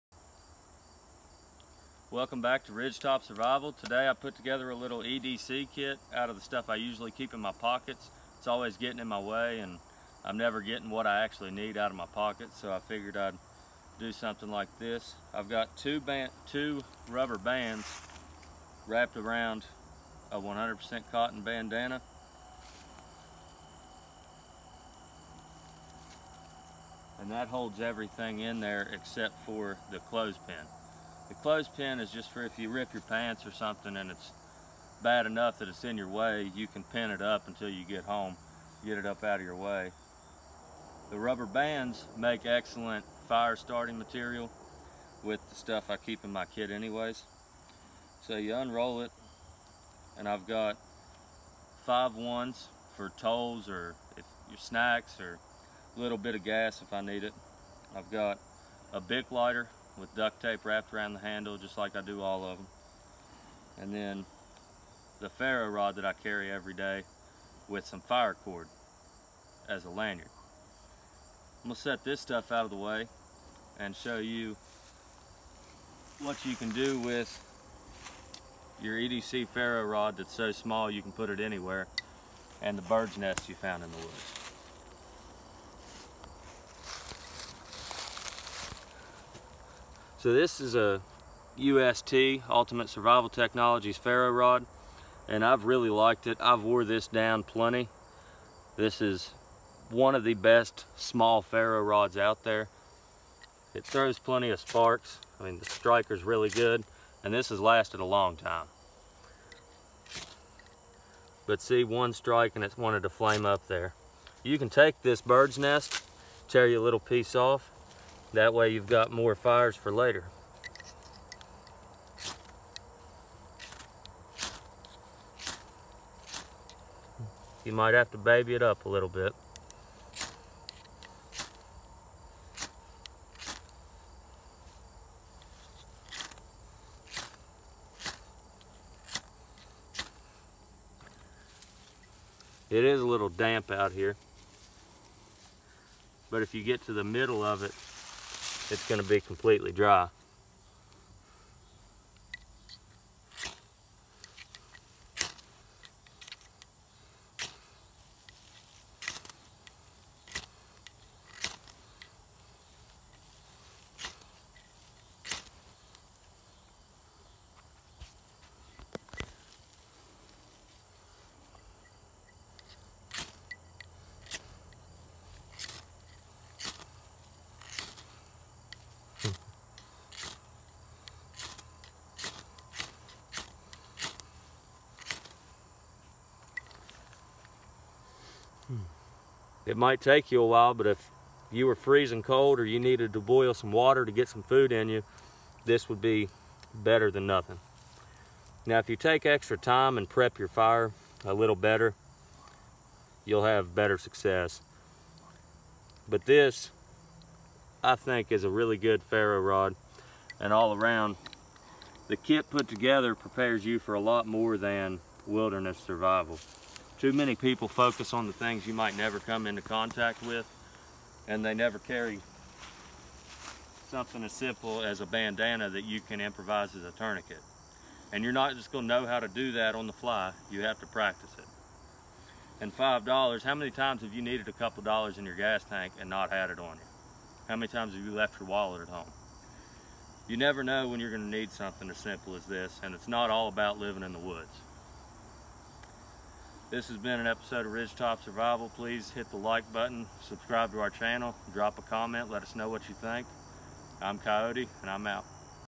EDC Roll - What do I carry?
On this episode of RidgeTop Survival Coyote shows off what he keeps in his EDC roll and talks about how it can be useful in day to day tasks.

UST SparkForce Fire Starter with Durable Construction and Lanyard for Camping, Backpacking, Hiking, Emergency and Outdoor Survival

BIC Lighter Classic, Full Size 12 Pieces, Bulk Packaging

Survival Bandana by OEM